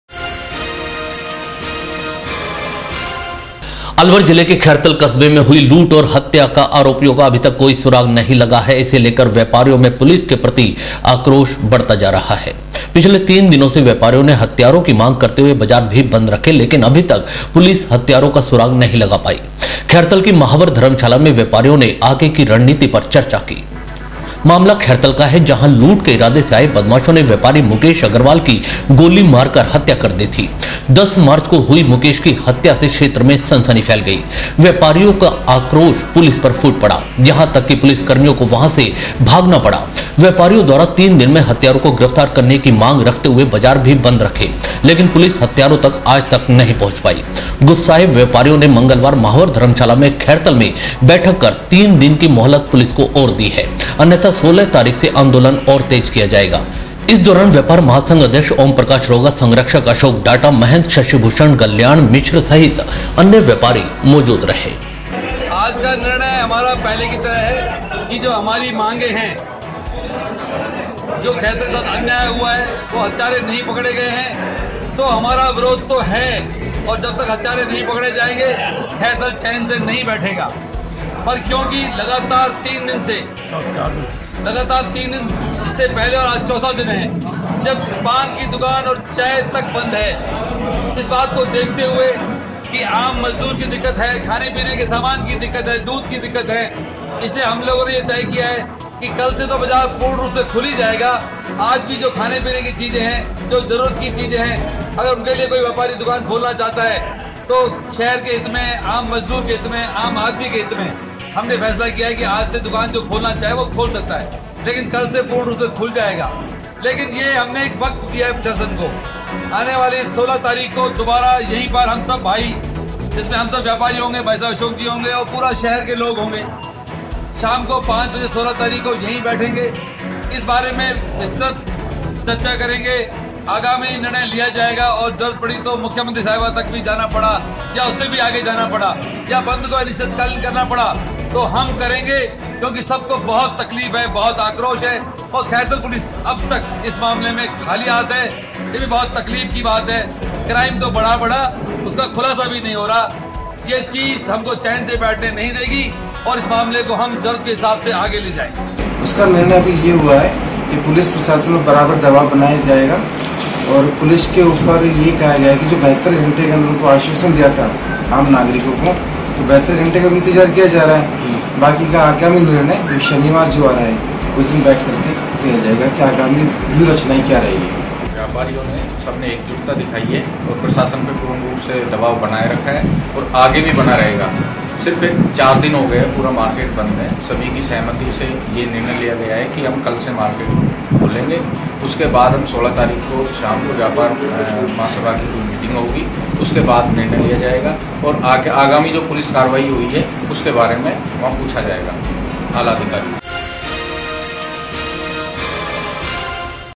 खैरथल में व्यापारियों की सभा
अलवर जिले के खैरथल कस्बे में हुई लूट और हत्या क आरोपियों का कोई सुराग नहीं लगा है। इसे लेकर व्यापारियों में पुलिस के प्रति आक्रोश बढ़ता जा रहा है। पिछले तीन दिनों से व्यापारियों ने हत्यारों की मांग करते हुए बाजार भी बंद रखे। लेकिन अभी तक पुलिस हत्यारों का सुराग नहीं लगा पार्ई। खैरथल की महावर धर्मशाला में व्यापारियों ने आगे की रणनीति पर चर्चा की।
मामला खैरथल का है जहां लूट के इरादे से आये बदमाशो ने व्यापारी मुकेश अग्रवाल की गोली मारकर हत्या कर दी थी। 10 मार्च को हई मुकेश की हत्या से क्षेत्र में सनसनी फैल गई। व्यापारियों का आक्रोश पुलिस पर फूट पड़ा। यहां तक की पुलिसकर्मियों को वहां से भागना पड़ा। व्यापारियों द्वारा तीन दिन में हत्यारों को गिरफ्तार करने की मांग रखते हुए बाजार बंद रखे। लेकिन पुलिस हत्यारों तक आज तक नहीं पहुंच पाई। गुस्साये व्यापारियों ने मंगलवार महावर धर्मशाला खैरथल में बैठक कर तीन दिन की मोहलत पुलिस को दी अन्यथा 16 तारीख से आंदोलन किया जाएगा। इस दौरान व्यापार महासंघ अध्यक्ष ओमप्रकाश रोघा, संरक्षक अशोक डाटा, महंत शशीभूषण गल्याण मिश्र सहित अन्य व्यापारी मौजूद रहै।